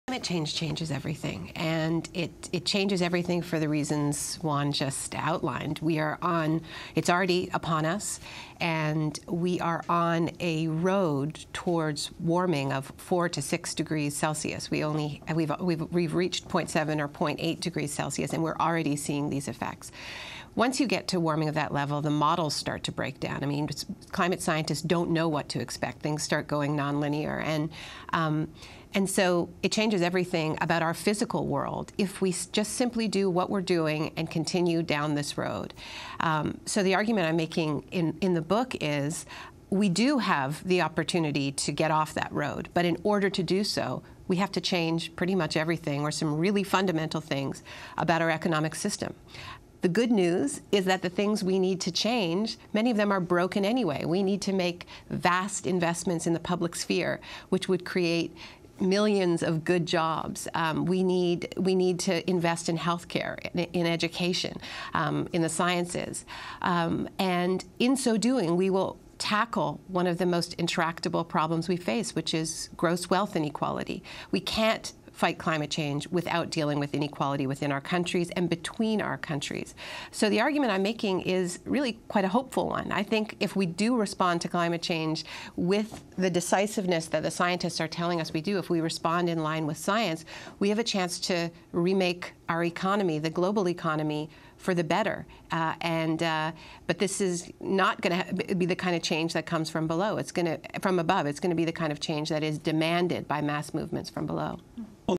Naomi Klein habla de los cambios necesarios para abordar la crisis ambiental
Mientras la ONU se prepara celebrar la cumbre mundial de un día sobre cambio climático, conversamos aquí con la galardonada autora Naomi Klein sobre su nuevo libro: "This Changes Everything: Capitalism vs. the Climate” (Esto cambia todo: el capitalismo contra el clima). Vea